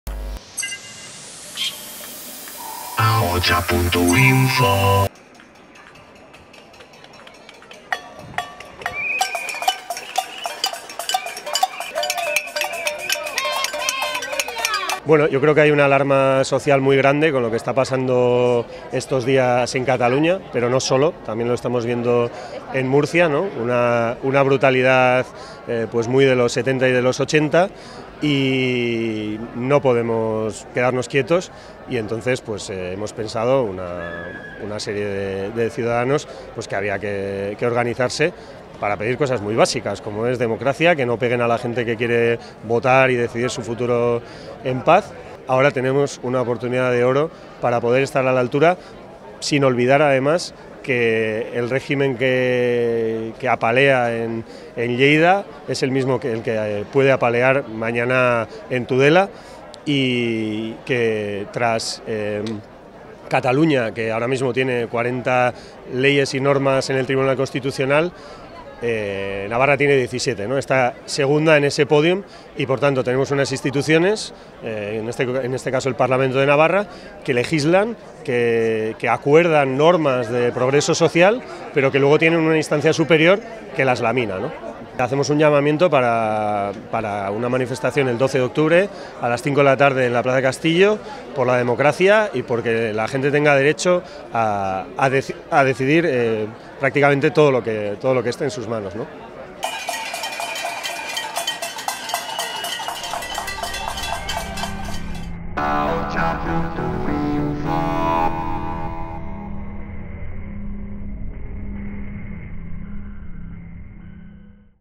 Cacerolada frente a la sede del Partido Popular
Una cacerolada ha tenido lugar este viernes frente a la sede del Partido Popular en Iruñea. Los asistentes han hecho sonar silbatos y cazuelas y coreado lemas en favor de Catalunya y del derecho a decidir. Asimismo, hacen una convocatoria de una manifestación por la democracia y el derecho a decidir que partirá el 12 de octubre de la Plaza del castillo a las 5 de la tarde